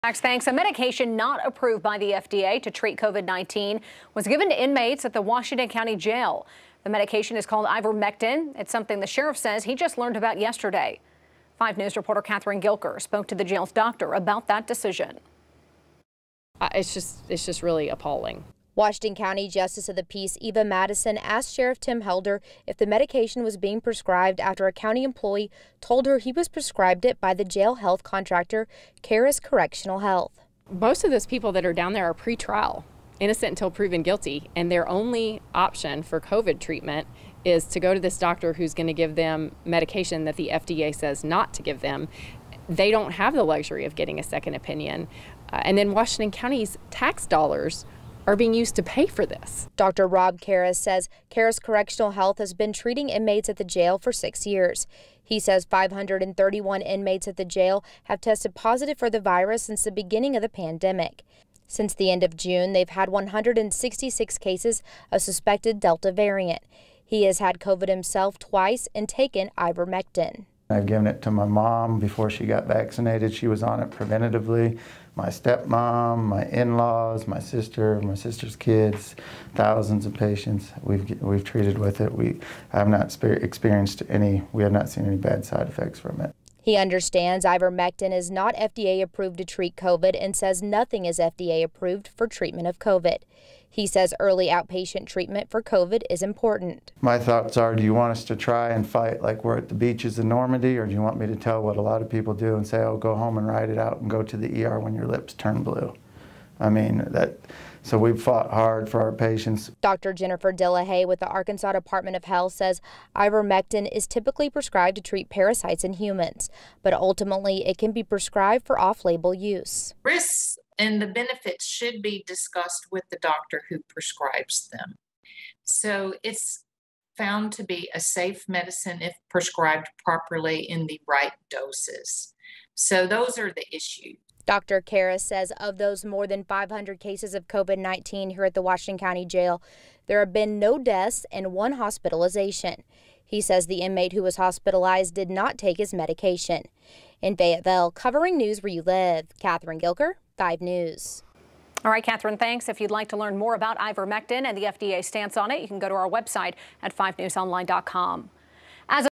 Washington County Jail gaining national attention after inmates receive anti-parasite drug to treat
The jail, along with a local doctor, is being questioned about using Ivermectin, a livestock deworming medication, to treat COVID-19 symptoms.